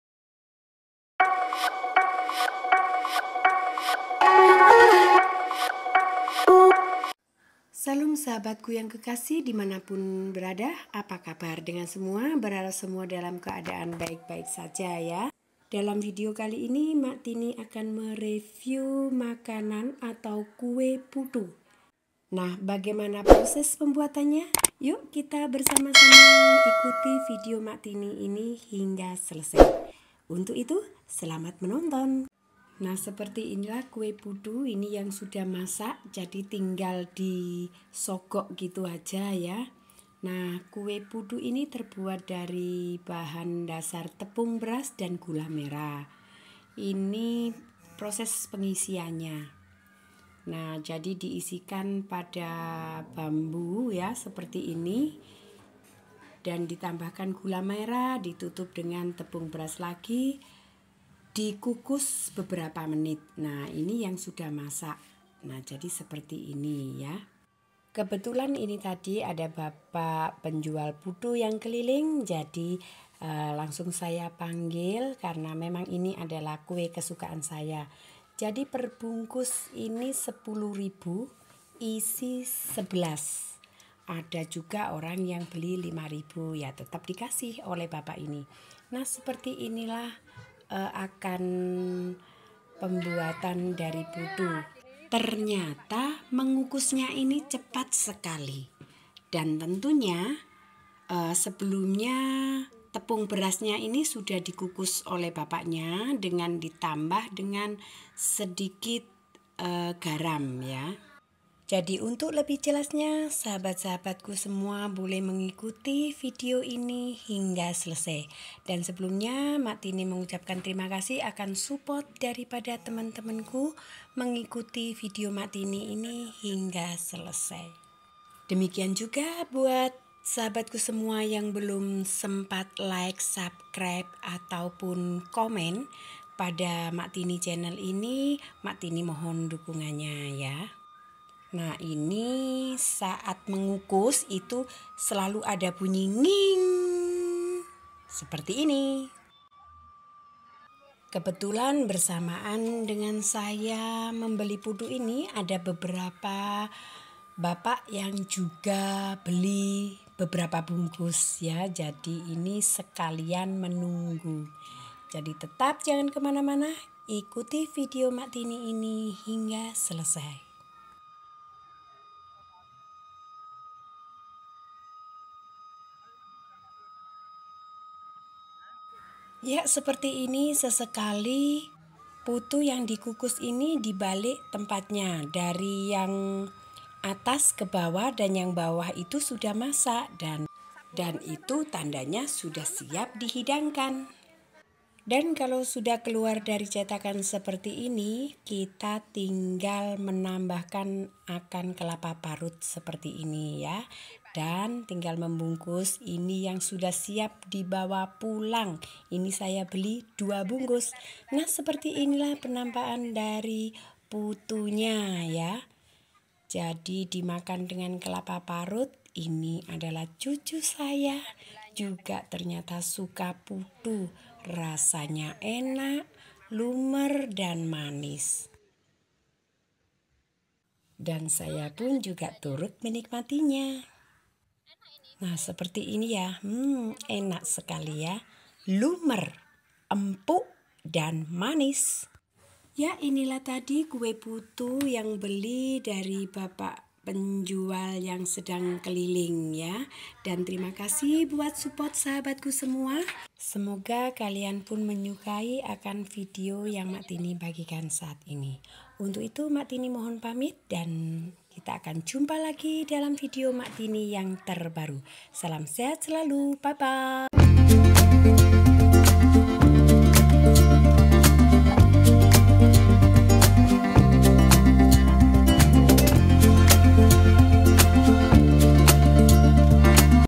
Kuliner Keliling Kue Putu@maktinichanel9748
Diskripsi : Putu yg terbuat dari bahan2 : Tepung beras,gula merah yg disisir,garam secukupnya,kelapa parut.
Hasilbdari putu yg telah masak ini sangat lembut,manis dan harum,pastix sangat enak@maktinichanel9748